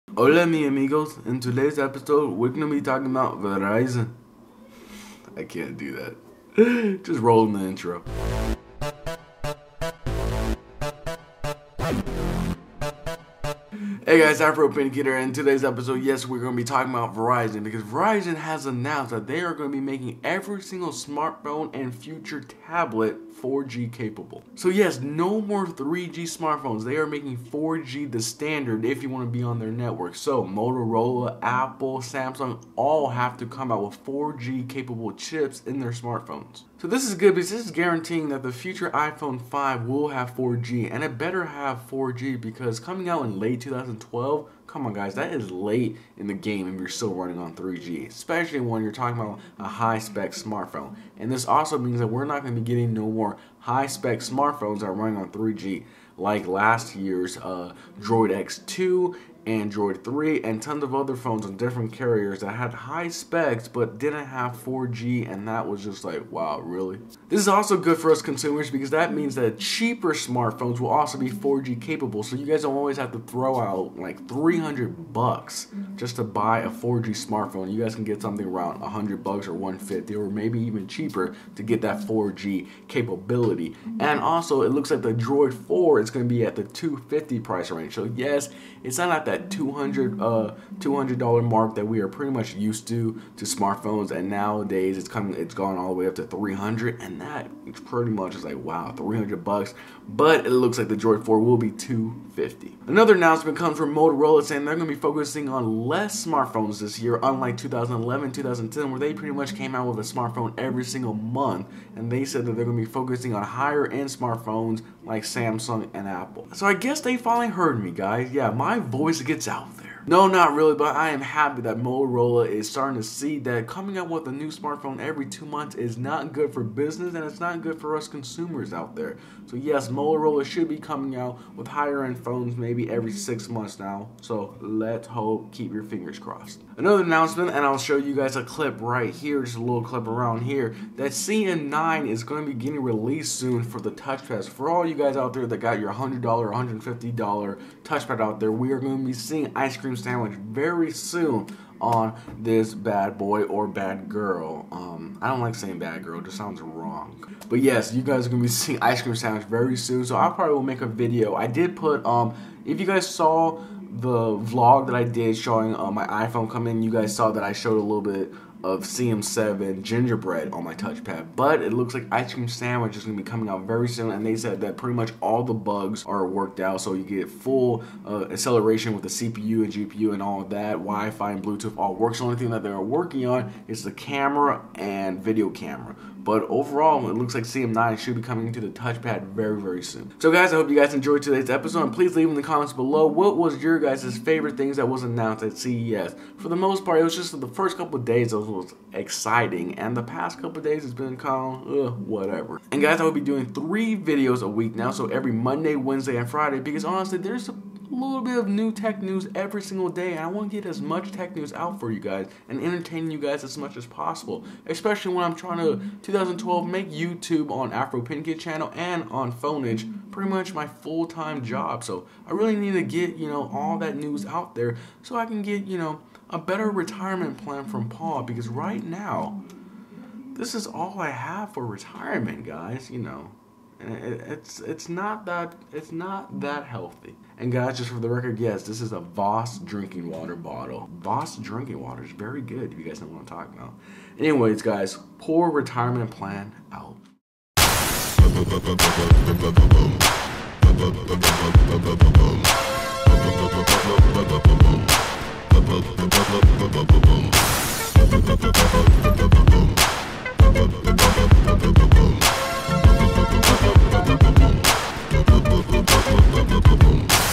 Verizon Announcement All Future Smartphones Will have 4G
All future verizon phones will be 4G LTE standard announced by Verizon. Also Motorola Has some announcements. Plus ice cream sandwich for touchpad.